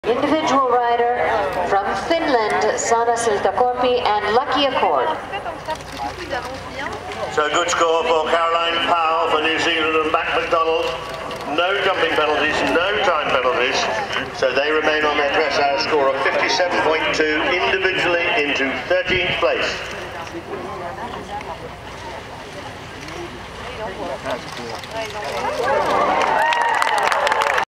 Peter Flarup DEN.AVI
The Land Between the Lakes (head of the lake complex) at the 2010 World Equestrian Games.